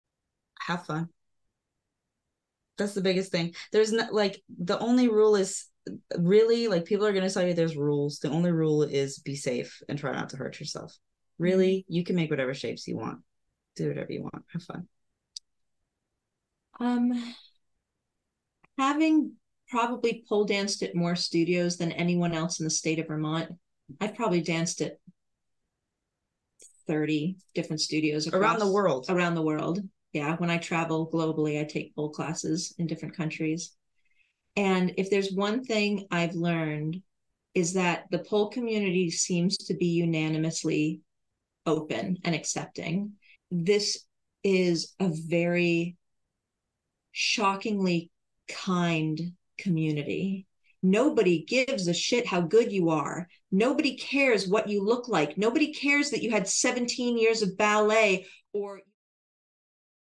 Savvie & Vickie Give Advice to Pole Dancers
Savvie & Vickie give their advice to pole dancers in this clip.

“Do whatever you want and have fun”

What advice would you give?

Get all the tips and more from two of the co-owners of @revol.vt in our latest podcast episode out now!